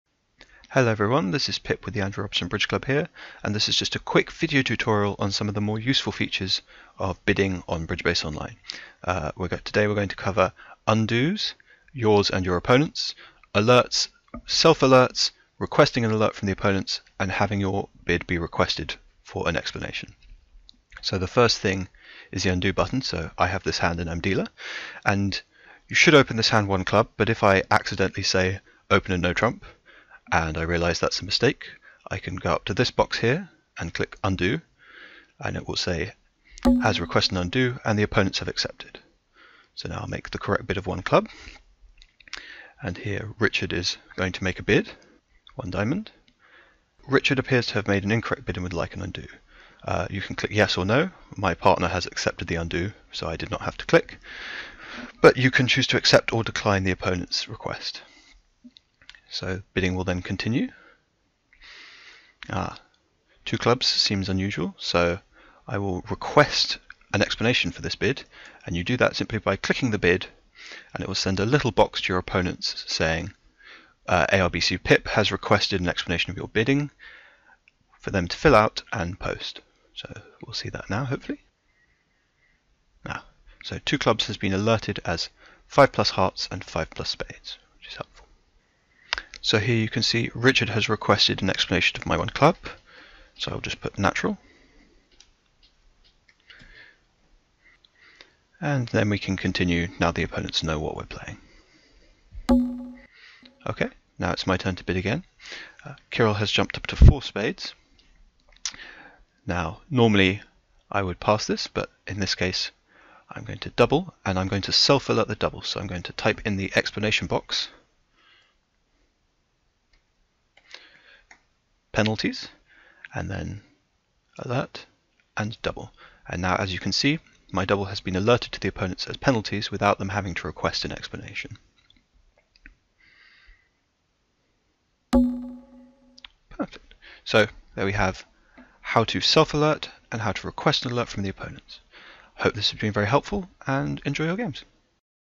How to Undo & Alert in Bridge Base Online
A quick video looking at the basics of how to use the Undo and Alert Functions on BBO.